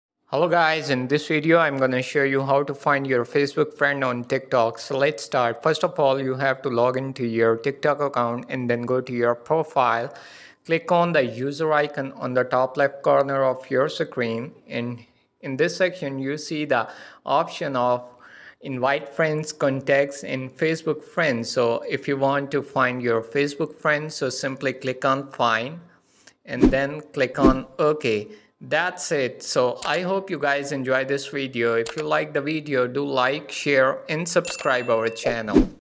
How to Find Facebook Friends on TikTok
In this Video, You'll Learn How to find facebook friends on tiktok.